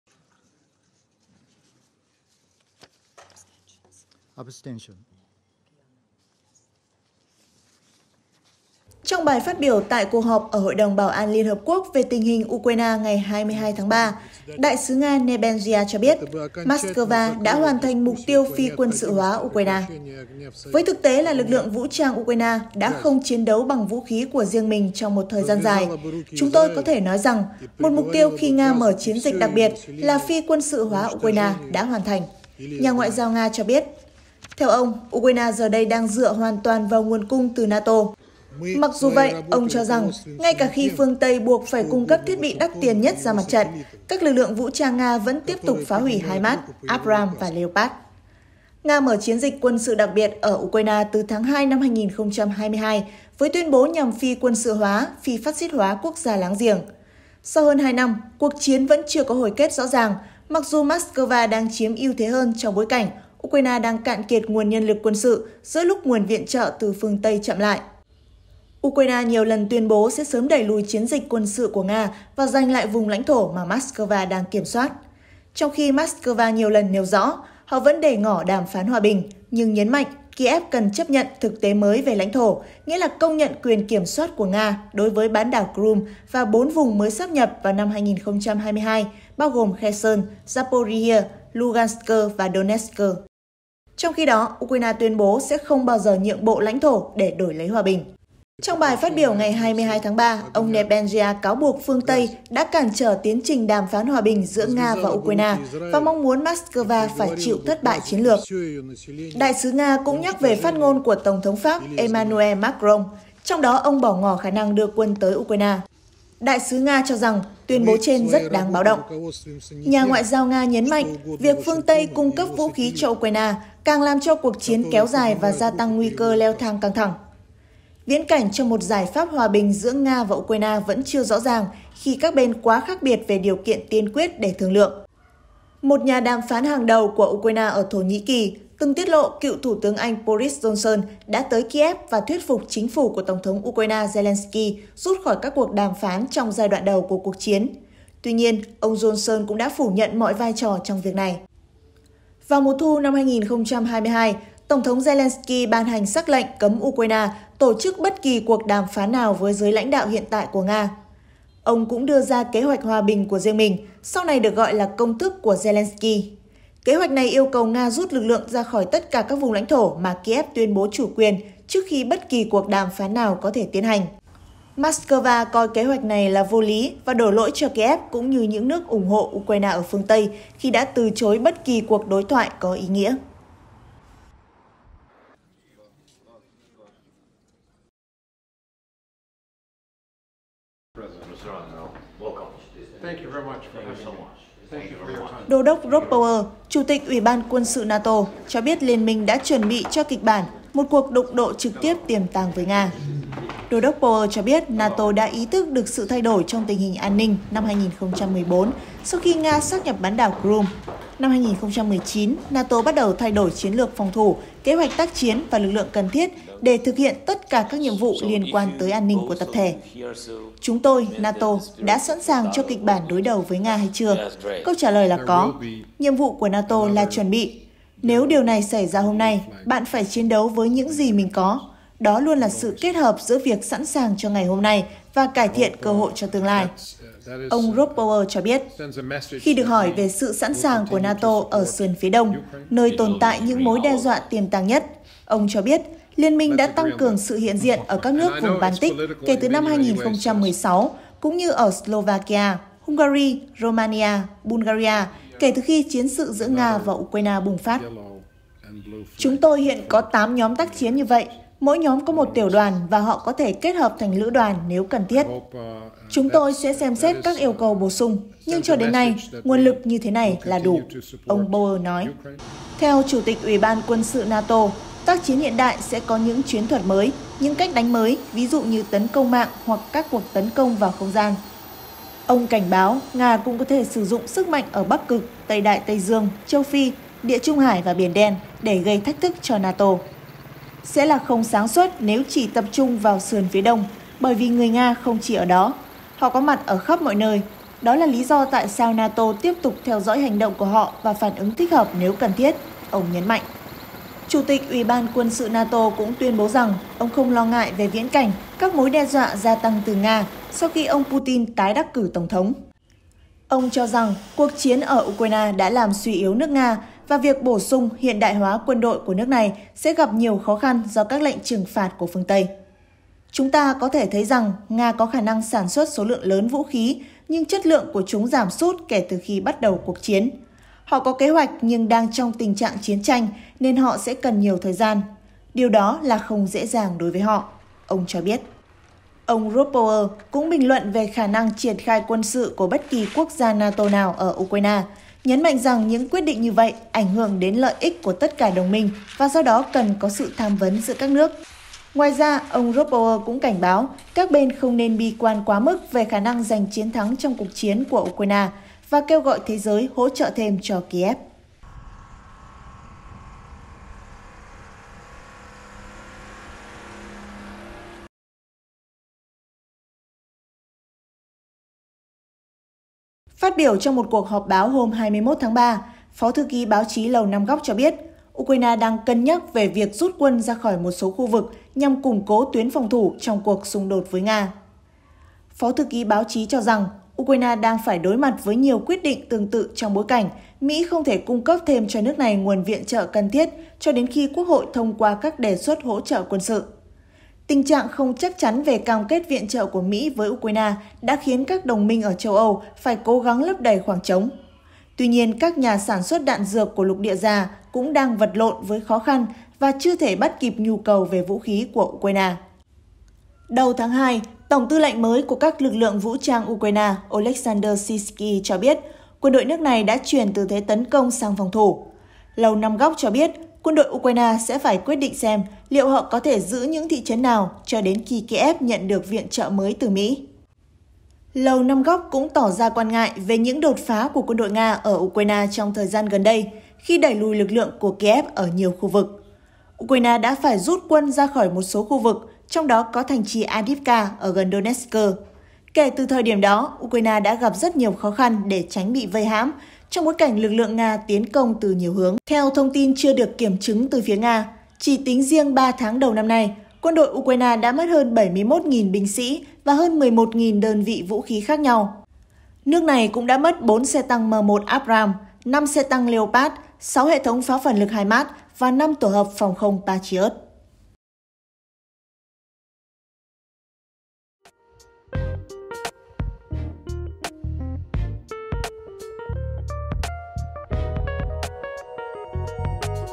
Nóng chiến sự: Nga tuyên bố đã hoàn thành mục tiêu "phi quân sự hóa" Ukraine | BÁO ĐIỆN TỬ VOV
BÁO ĐIỆN TỬ VOV | Đại sứ Nga tại Liên hợp quốc Vassily Nebenzia cho biết nước này đã hoàn thành mục tiêu phi quân sự hóa Ukraine mà họ đề ra khi bắt đầu chiến dịch quân sự vào tháng 2/2022.

37 Bà Triệu, Hoàn Kiếm, Hà Nội, Việt Nam